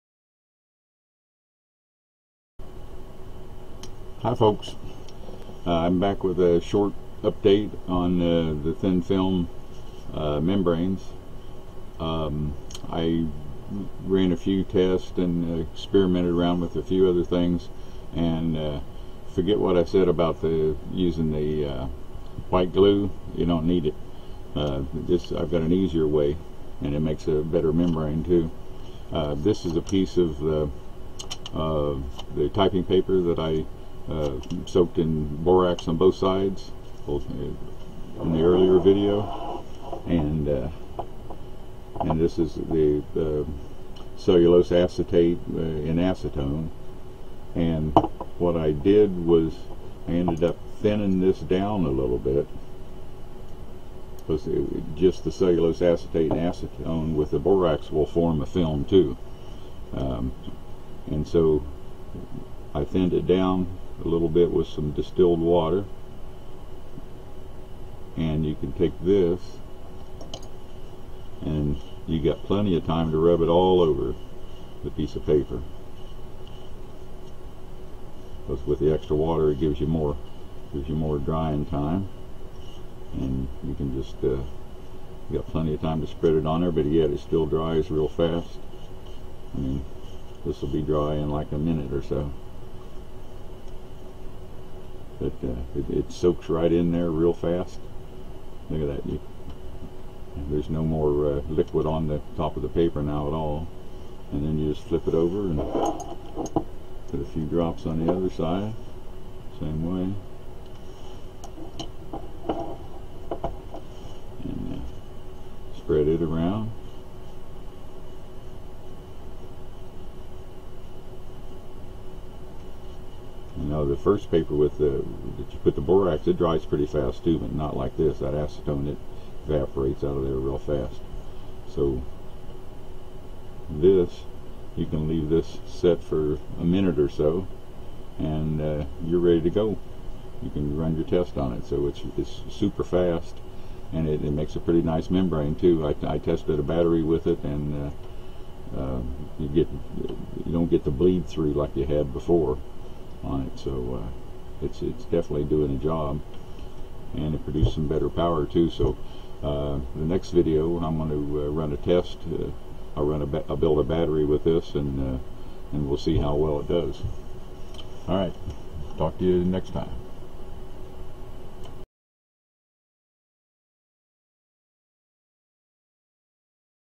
Thin Film Borax Separator #2
An easier version that gives more time to apply the thin film.